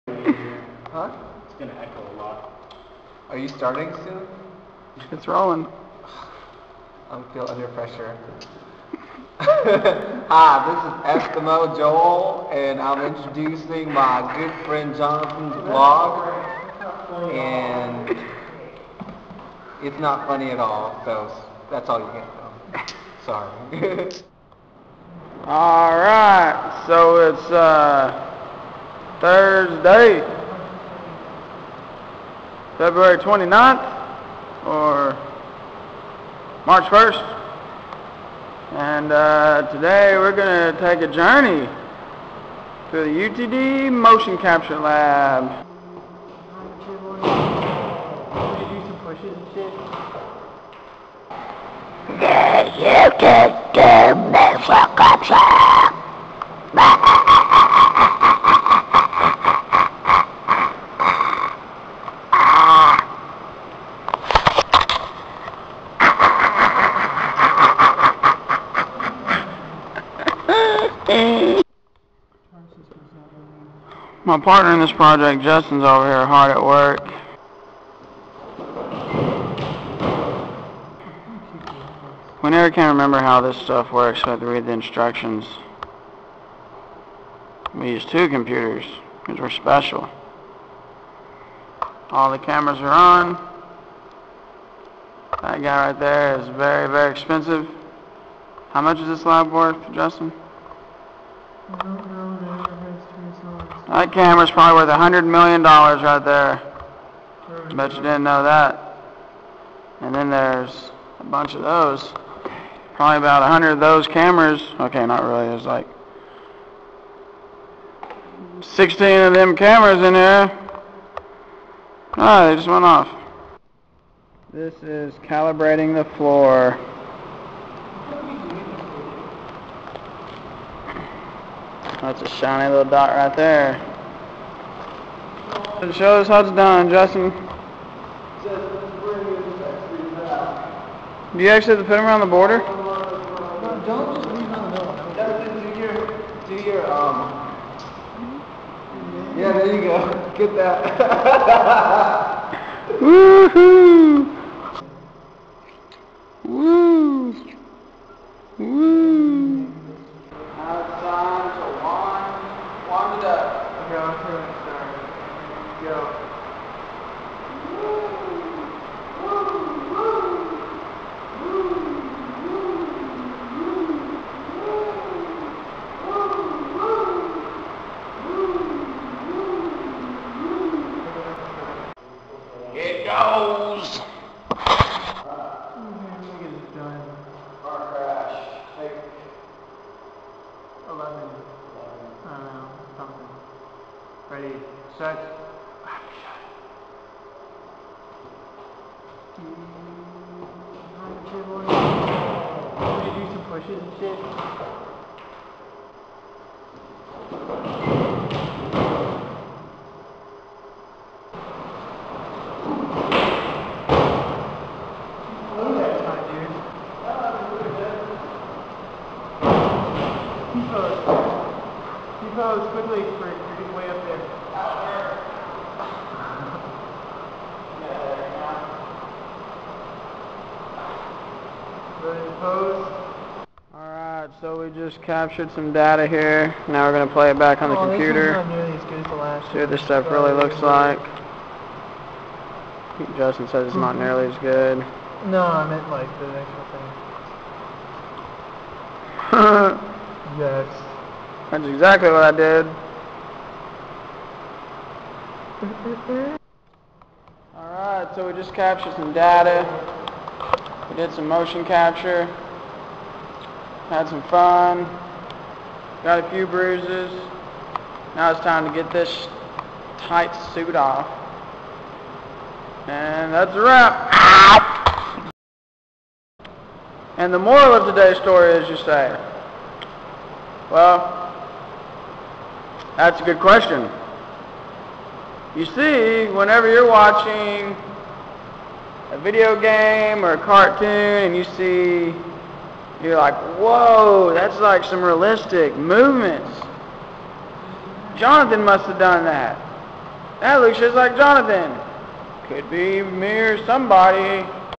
This is how the mo cap goes down
Decided  to do an extended entry on a day of life in the motion capture suit. ...it hurts. REALLY bad.